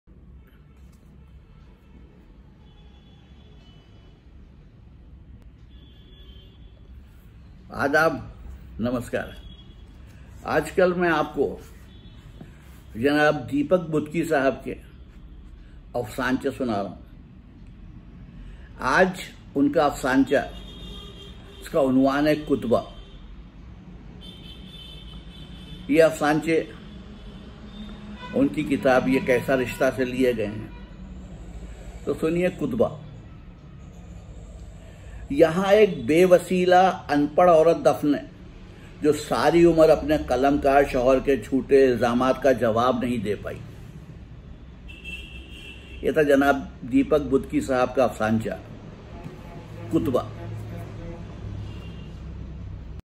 Kutba by Deepak Budki
Deepak Budki

 Deepak BudkiDeepak Budki is a renowned Urdu short story writer. He started writing short stories in 1971. His first short story "Salma" was published in the Daily Hamdard, Srinagar. One of his short stories 'Reyzey' was televised by Doordarshan Srinagar. In 1976 he qualified for Indian Postal Services and thereafter gave up writing short stories for almost twenty years. 1996 saw the rebirth of the writer., ever since he has published more than sixty short stories which have been published in the leading magazines of Indo-Pak subcontinent, United Kingdom and other countries where Urdu is read and understood. His short stories have been translated in Hindi, Telugu and other countries where Urdu is read and understood. His short stories have been translated in Hindi, Telugu and Kashmiri as well. His two collections of short stories, 'Adhoore Chehre' and 'Chinar Ke Panje' have been published in 1999 and 2005 while “Zebra crossing per Khada Adami' is expected to be released shortly. Budki is a big name in Urdu literature. A few leading magazines have brought out special issues in his honour.